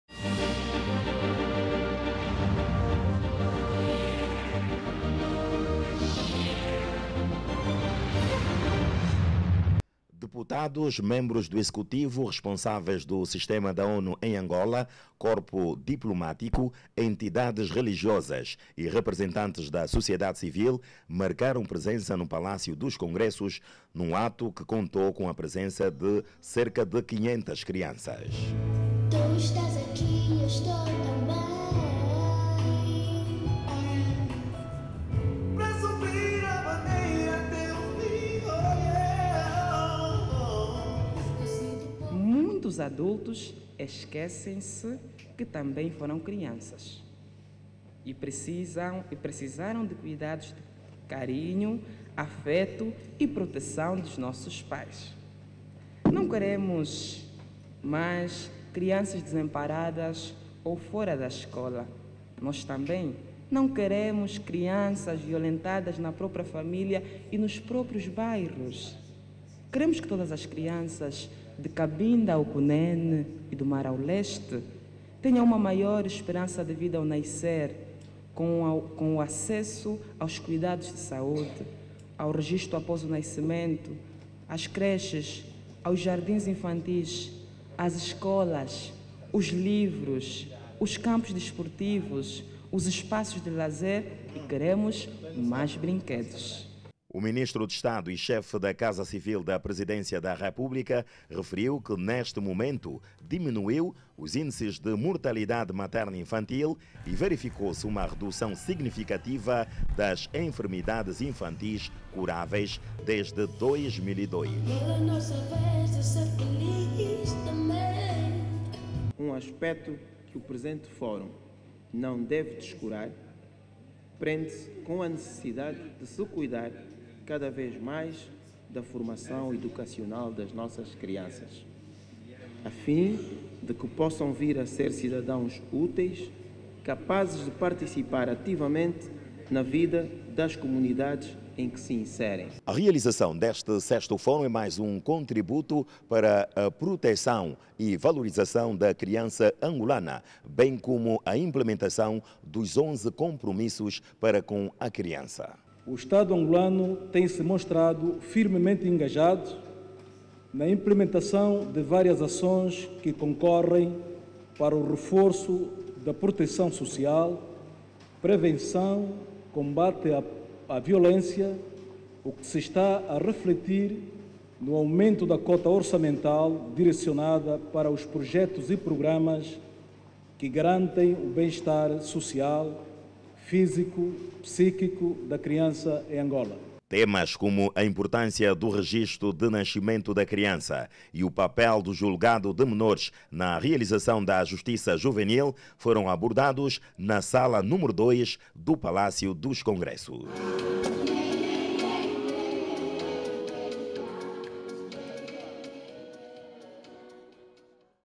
Luanda:Executivo mais engajado para o bem-estar social da criança angolana | TV Zimbo
O Executivo angolano está a aumentar o dinheiro para apoio e projectos e programas que garantam o bem-estar social da criança angolana.

Esta constatação foi feita pelo presidente do Conselho Nacional da Criança, João Baptista Kussumua, durante o VI fórum Nacional da Criança que decorre no Palácio dos Congressos